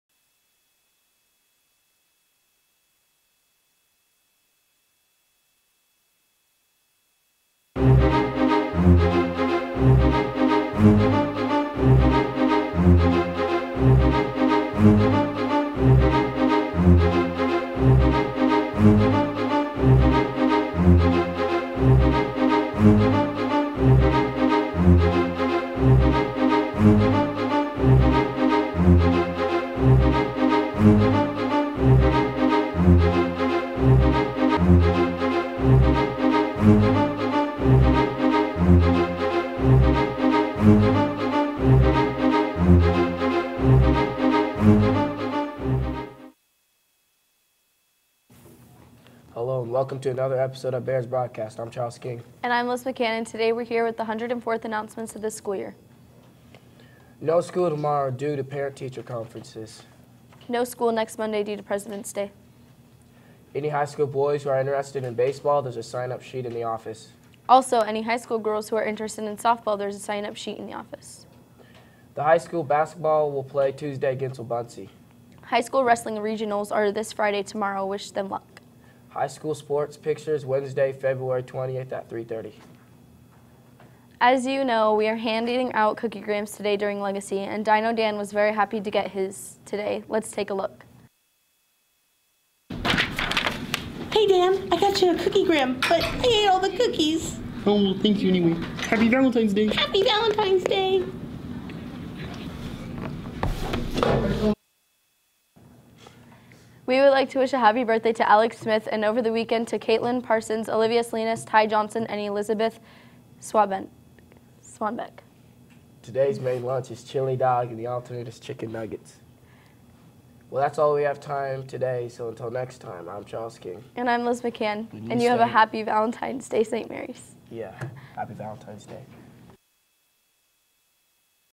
Bears Broadcast 2-14-19
Dino Dan enjoys a Valentine's surprise.